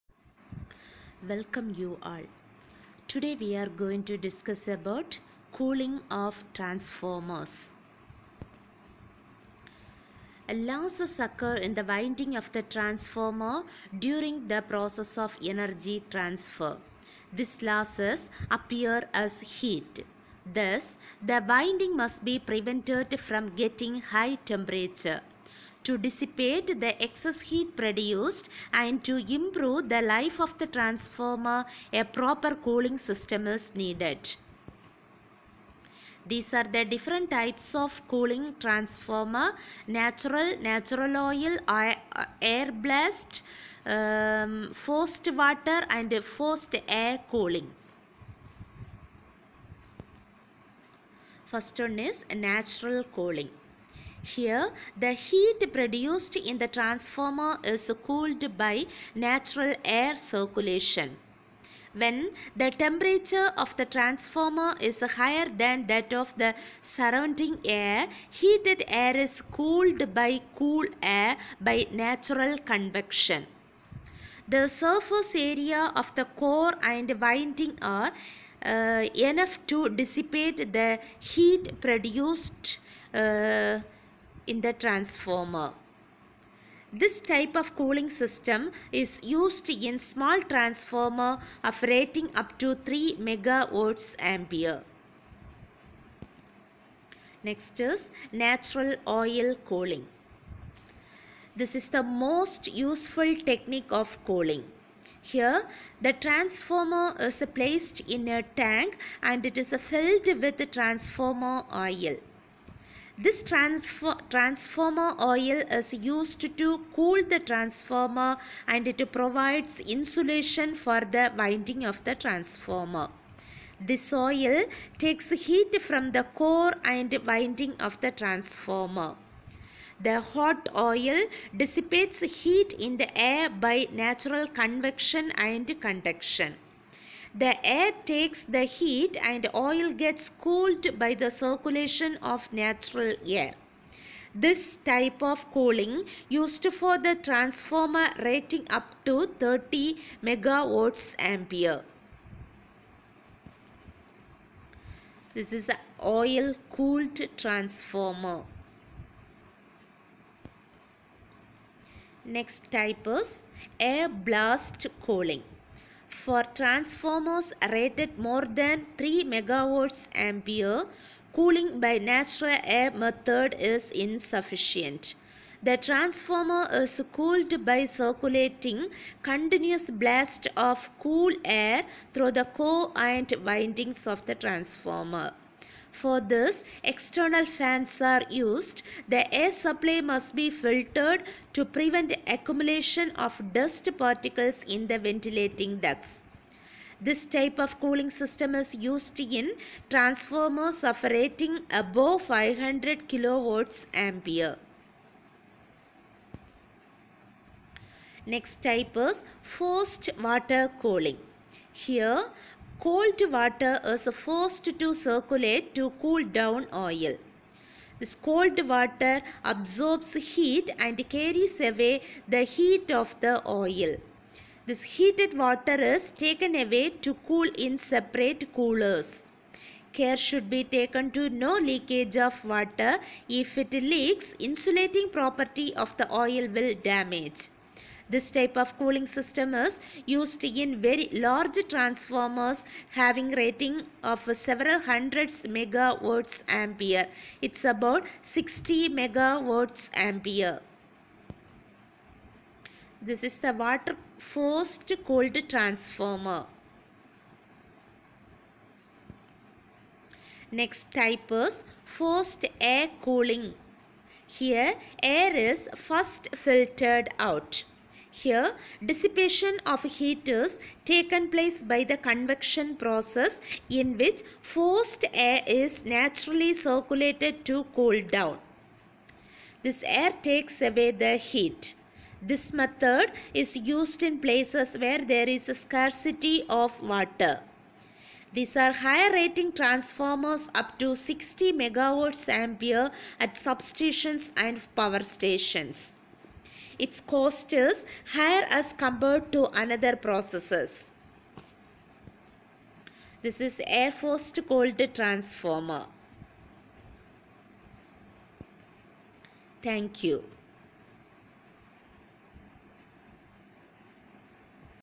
COOLING OF TRANSFORMERS / II B.Sc. / PHYSICS
COOLING OF TRANSFORMER PLAYS AN IMPORTANT ROLE TO IMPROVE THE LIFE OF THE TRANSFORMER / Natural cooling / Natural oil cooling / Air blast cooling / Forced water cooling  / Forced air cooling /